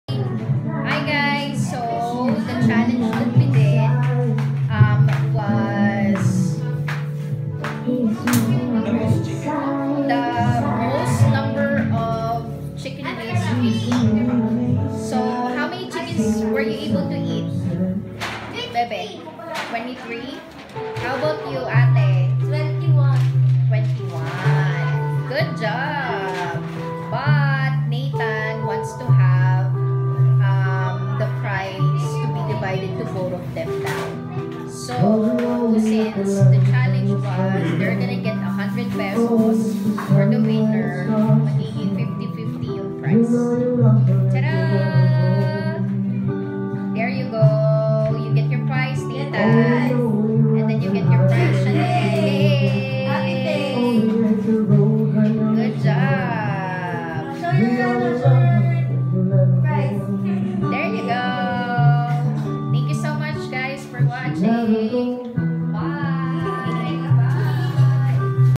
Unli Chicken Wings Challenge Part 4 (Who's the winner?)
Sinabi ni nathan na hatiin nalang daw yung price. Super bait na bata.